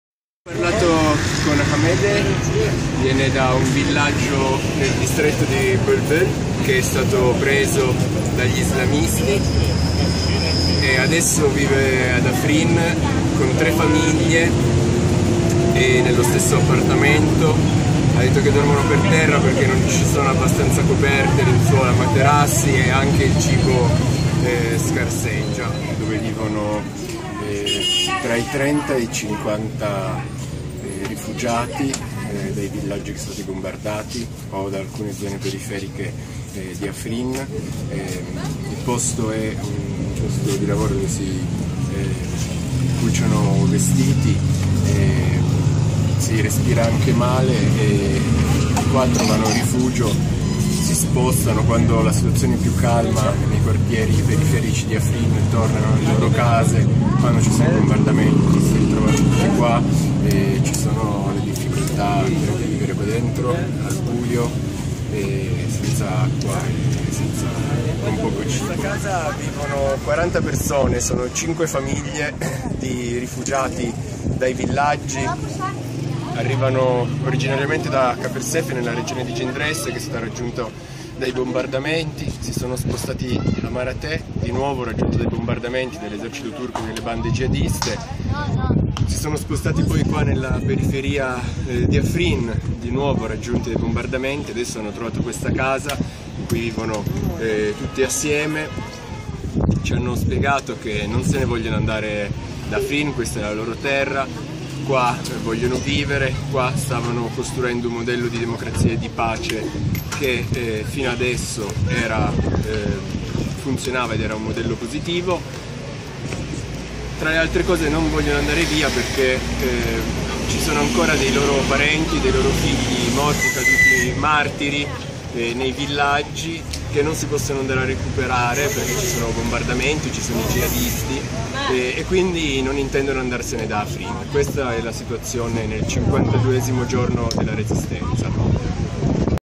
Afrin: nei rifugi degli sfollati siriani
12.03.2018: un nuovo video inviatoci da Jacopo e girato nei rifugi degli sfollati siriani nella città di Afrin, a rischio bombardamento dalla Turchia che assedia la città in queste ore.